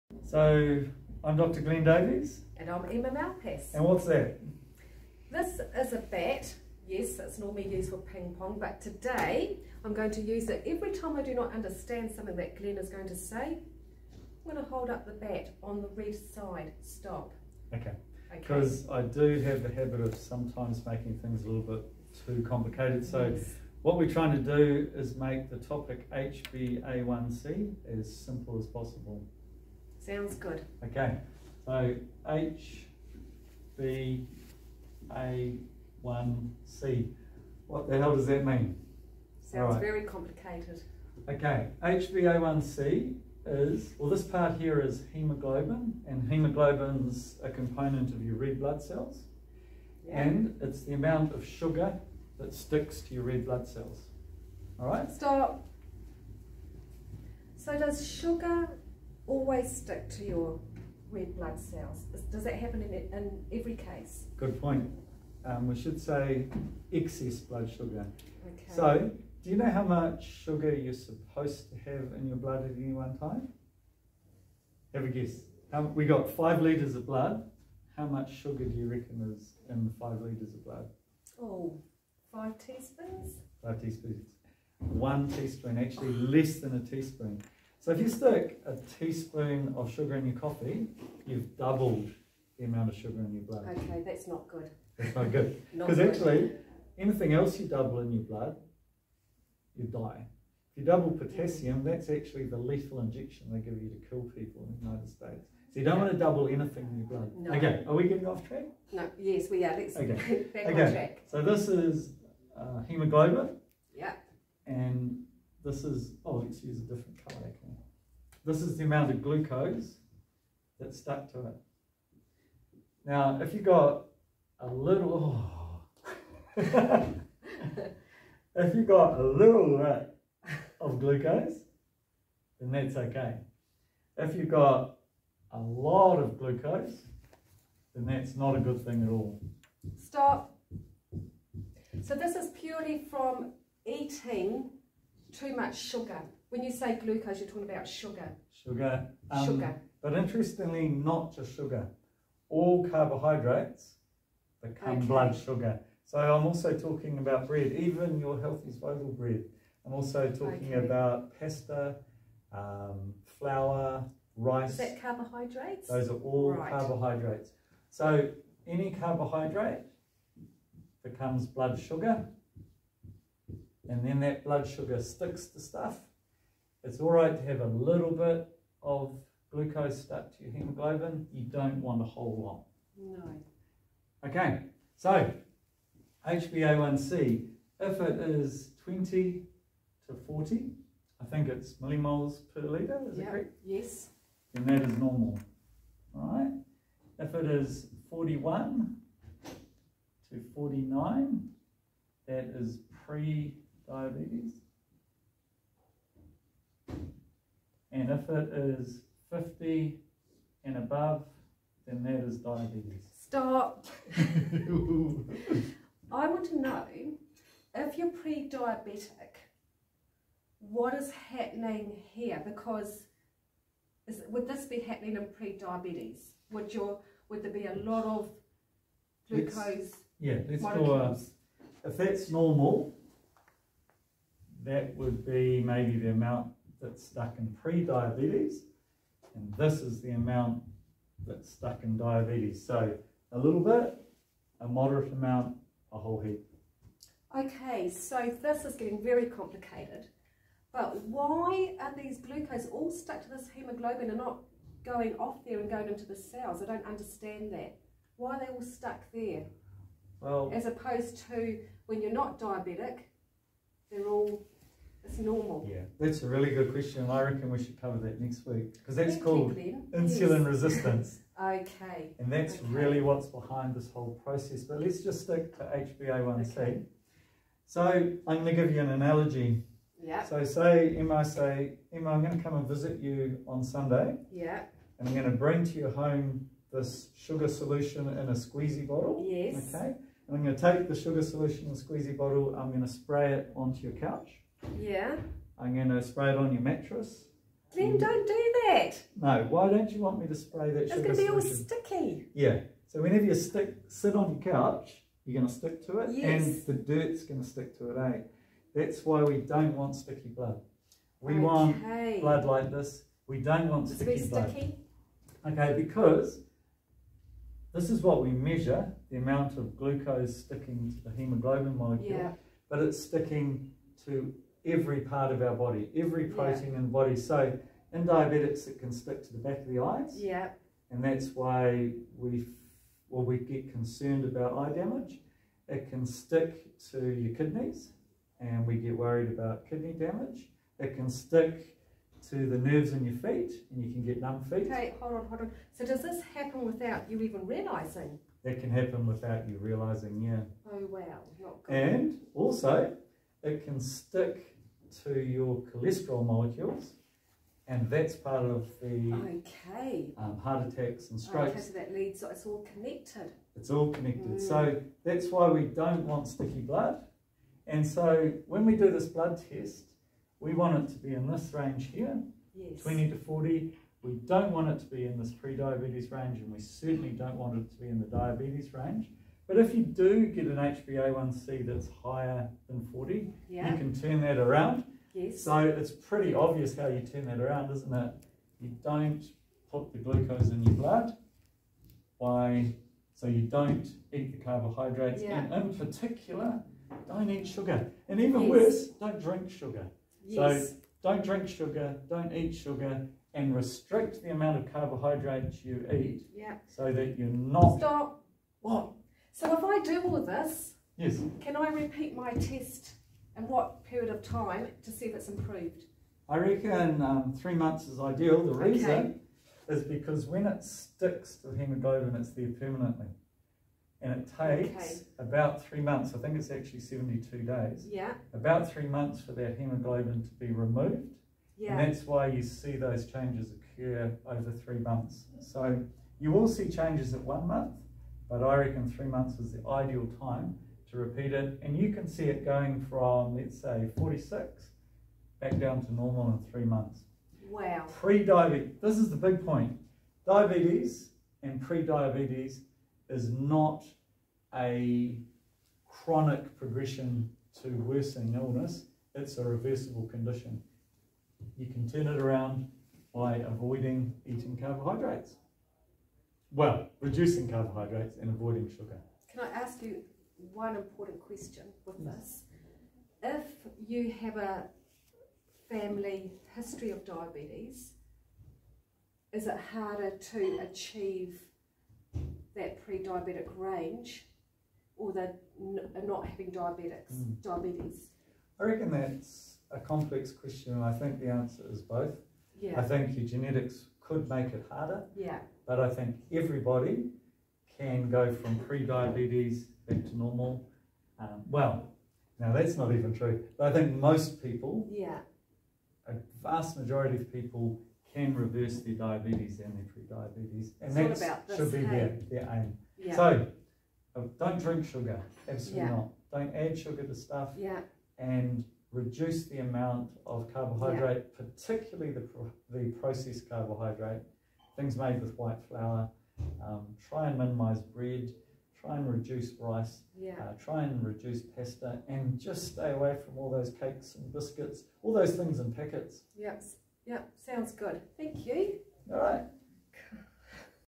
HbA1c made simple
HbA1c made simple. You don't want sticky blood. HbA1c measures the amount of sugar stuck to your cells. This sugar can cause damage to the things it sticks to.